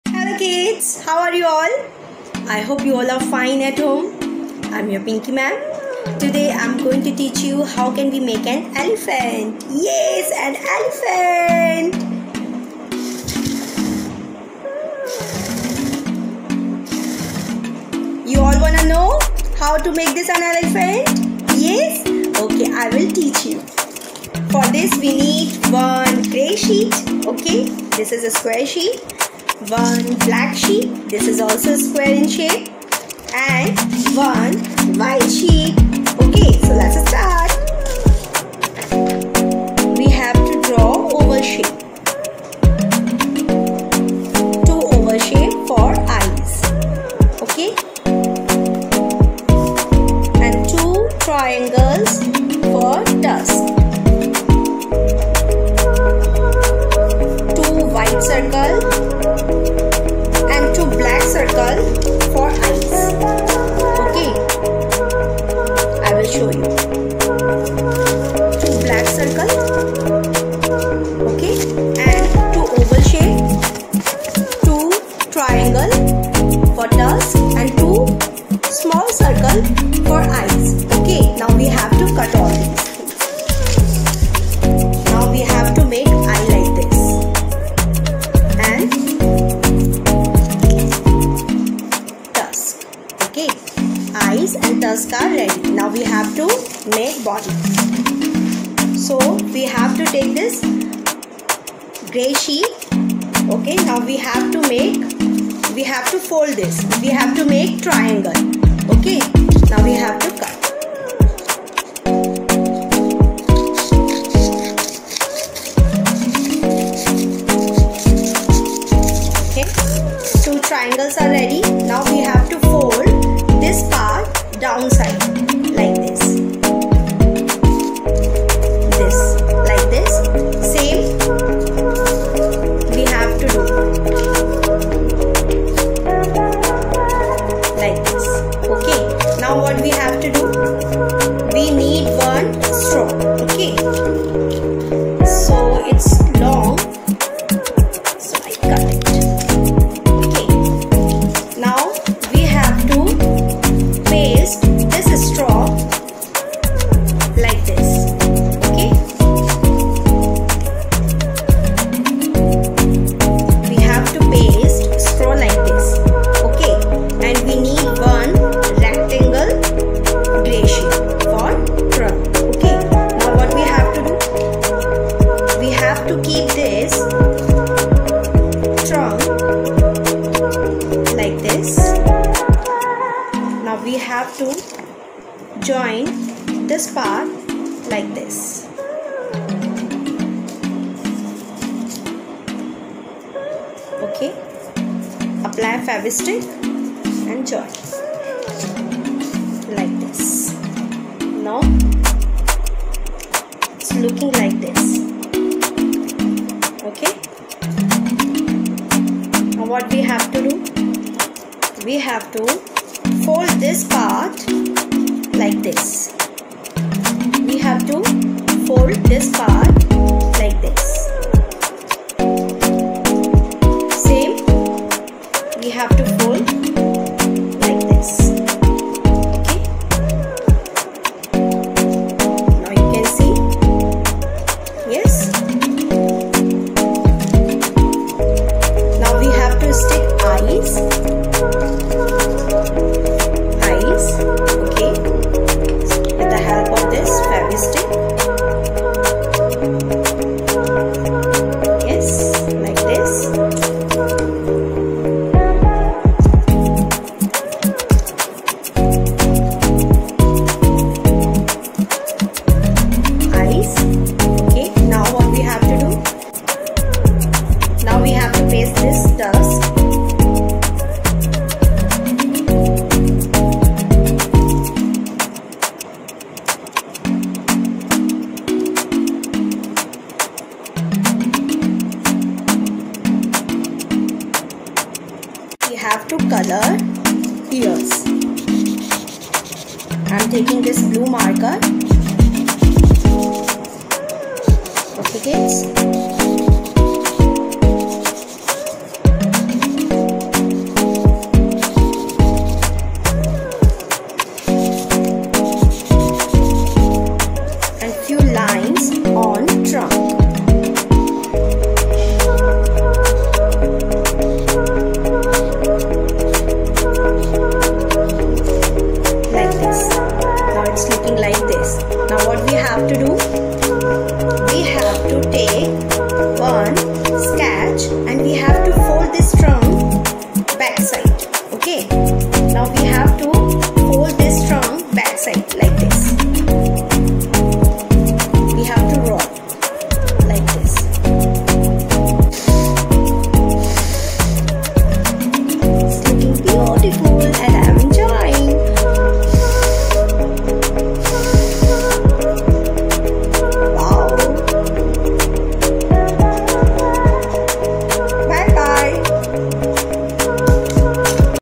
Art and craft:-🐘 Elephant with moving trunk.
Grey, White and Black sheet
Straw
Scissors
Blue marker
Fevicol/Fevistick